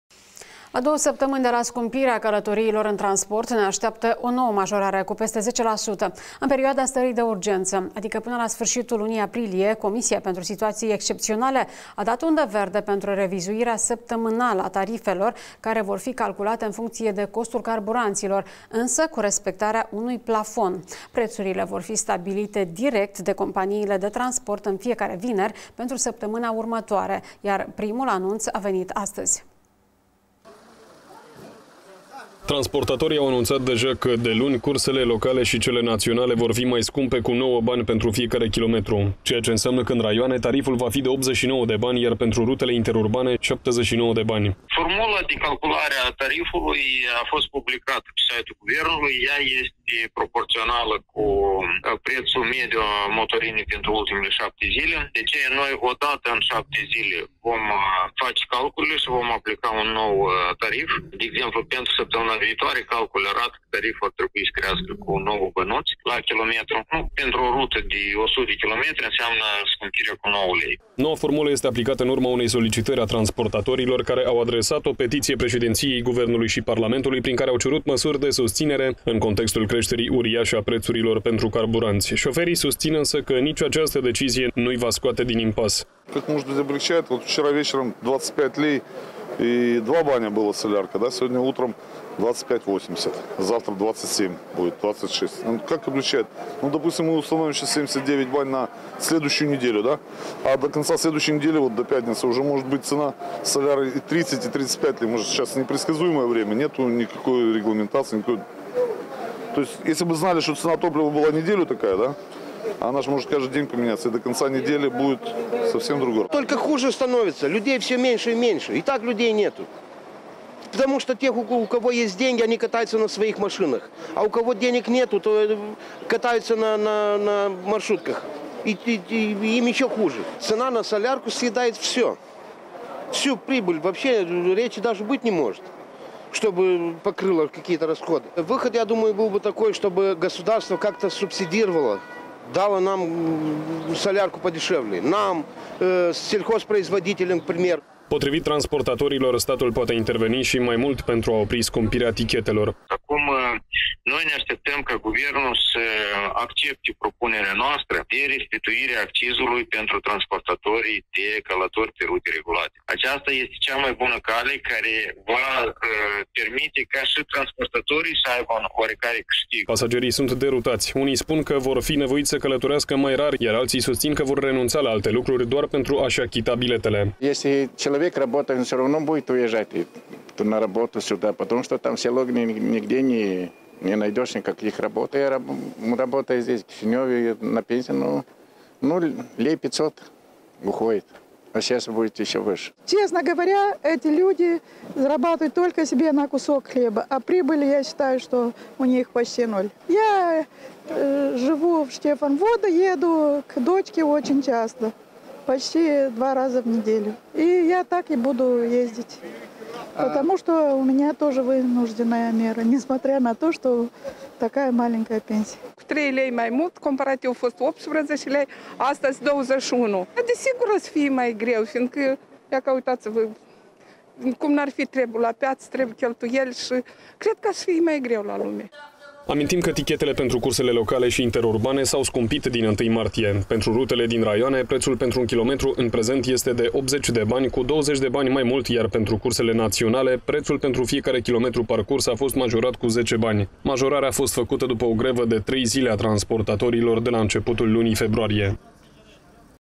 Călătoriile se vor scumpi săptămânal
La două săptămâni de la scumpirea călătoriilor în transport, ne așteaptă o nouă majorare - cu peste 10 la sută. În perioada stării de urgență, adică până la sfârșitul lunii aprilie, Comisia pentru Situații Excepționale a dat undă verde pentru revizuirea săptămânală a tarifelor, care vor fi calculate în funcție de costul carburanților, însă cu respectarea unui plafon. Prețurile vor fi stabilite direct de companiile de transport în fiecare vineri pentru săptămâna următoare, iar primul anunț a și venit azi.